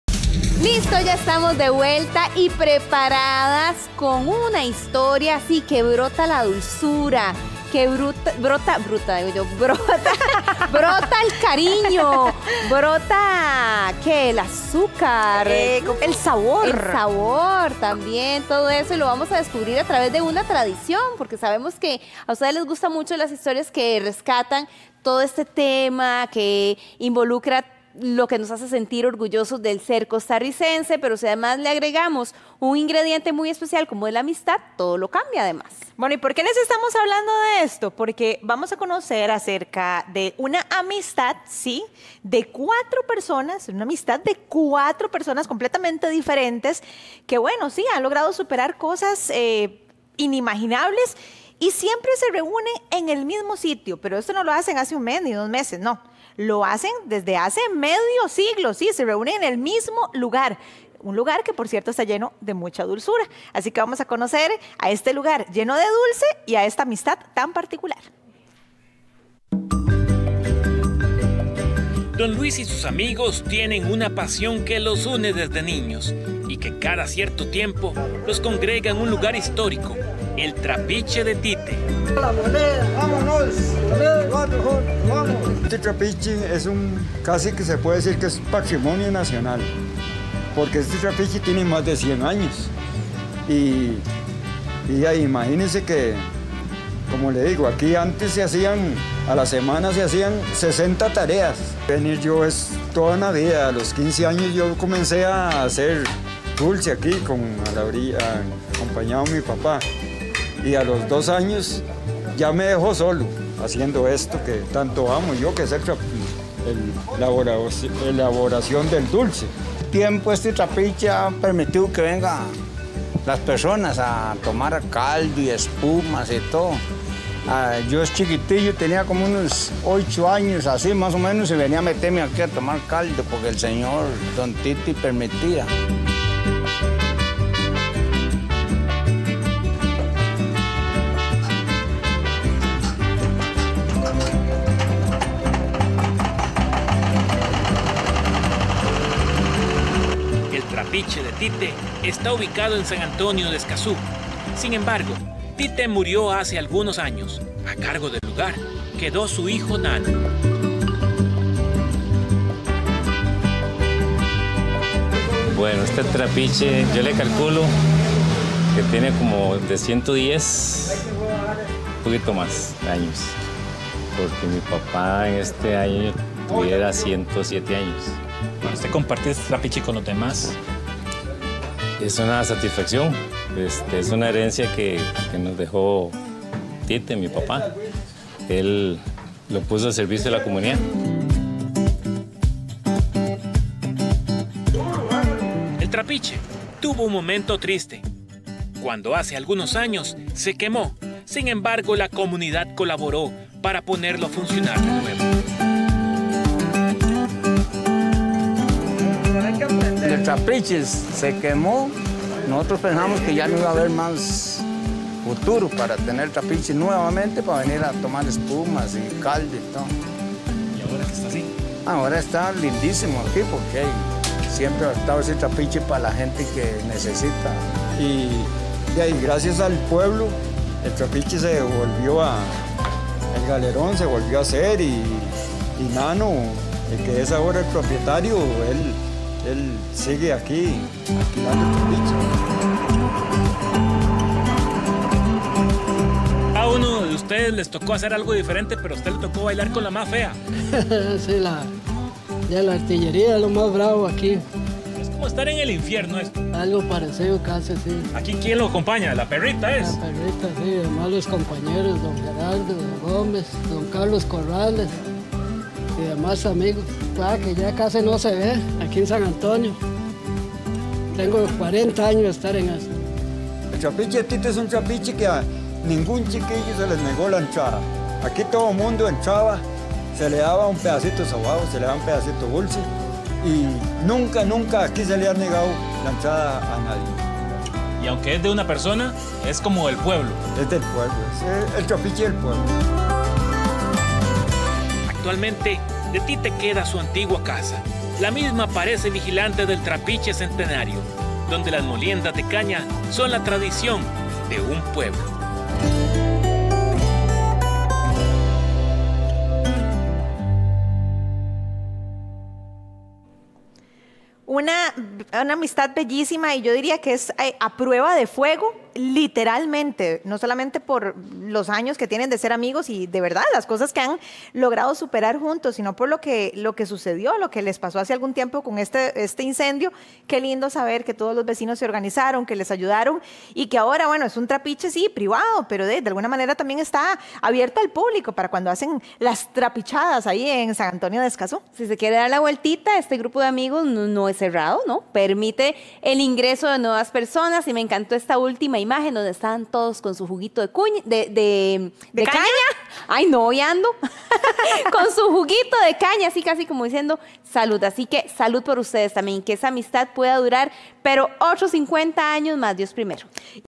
Este trapiche tiene 50 años de pura historia
Desde que eran niños, estos amigos tienen la costumbre de reunirse en torno al trapiche para compartir historias y alegrías.
El trapiche ya tiene más de 100 años y cuenta con cariño y apoyo de su comunidad.